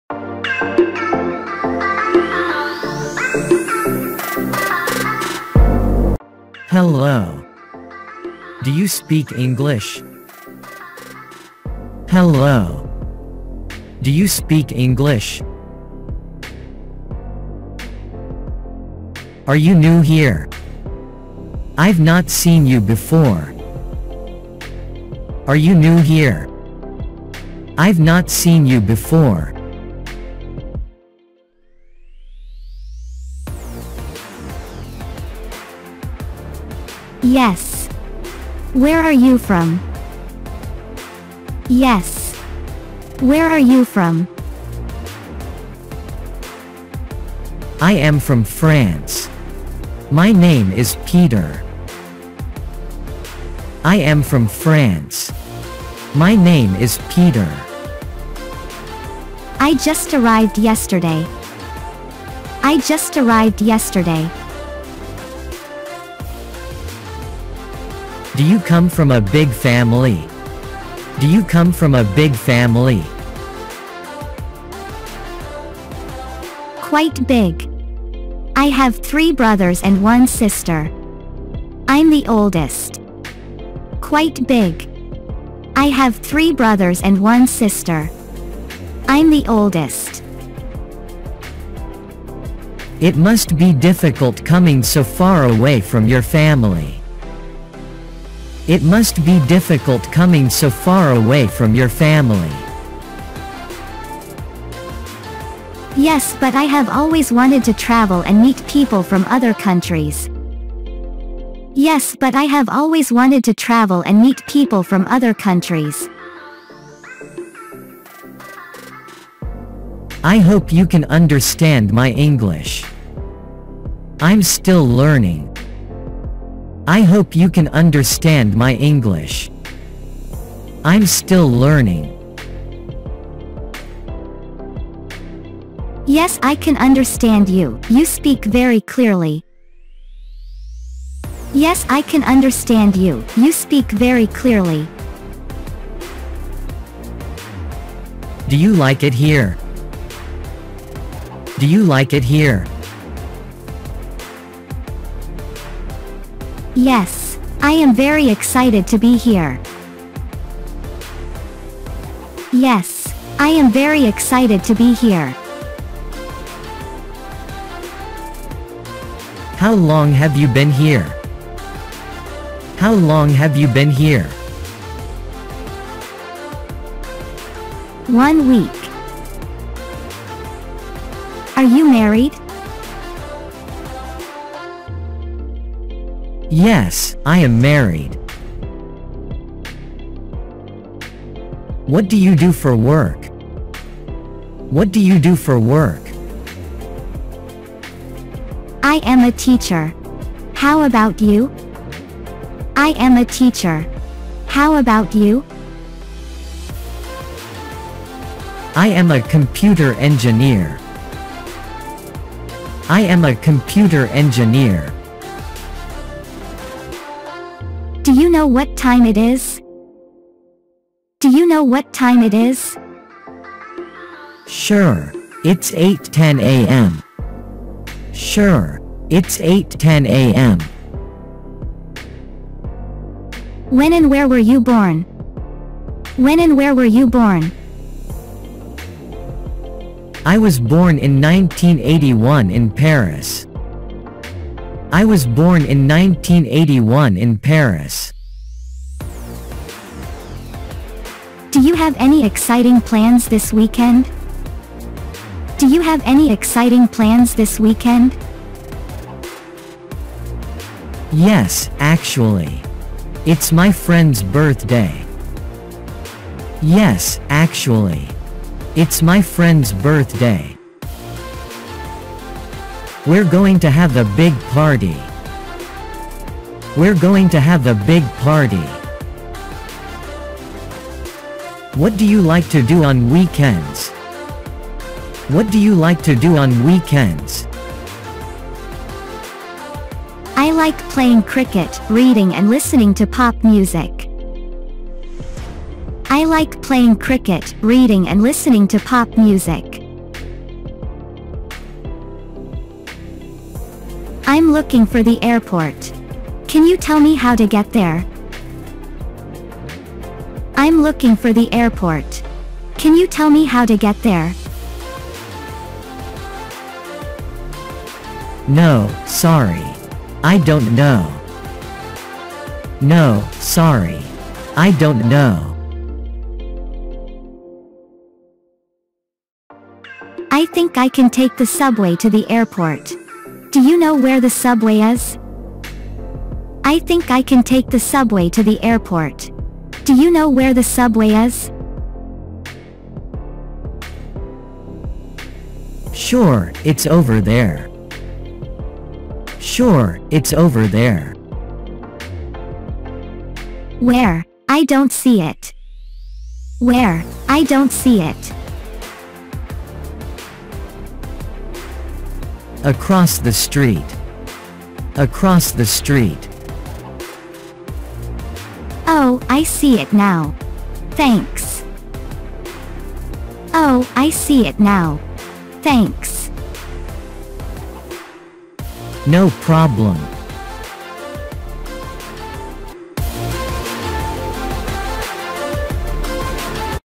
Daily English Conversation / Meeting new partners in daily speaking
Learn to speak English in daily speaking, meeting new partner in daily speaking.